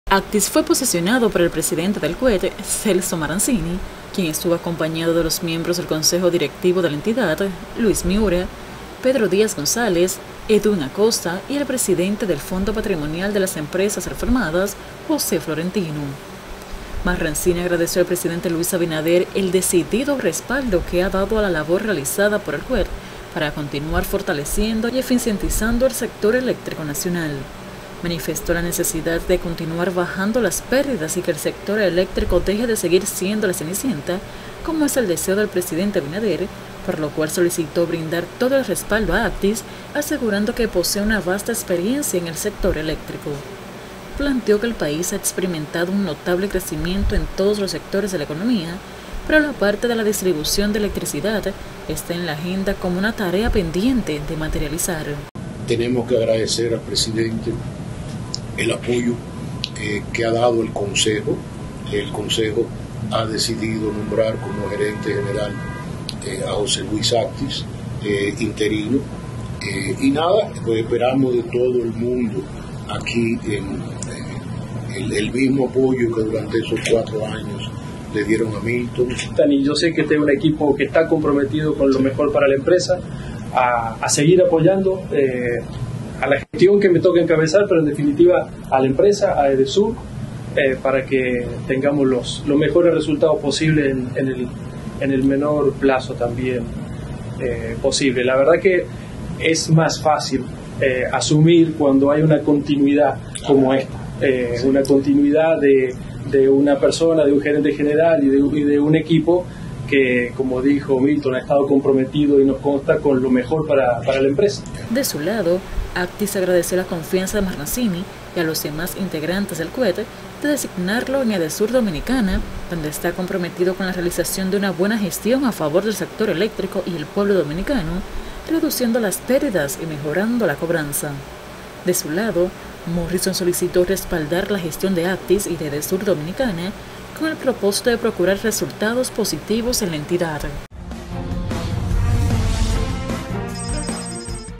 Presidente de CUED Celso Marranzini posesiona a nuevo Gerente General de Edesur Dominicana
El Consejo Unificado de las Empresas Distribuidoras de Electricidad (CUED) designó como Gerente General Interino de Edesur Dominicana al economista José Luis Actis, quien se comprometió a continuar apoyando una eficiente gestión para procurar los mejores resultados en el menor plazo posible, con un trabajo en equipo y dando continuidad a la labor realizada por su antecesor.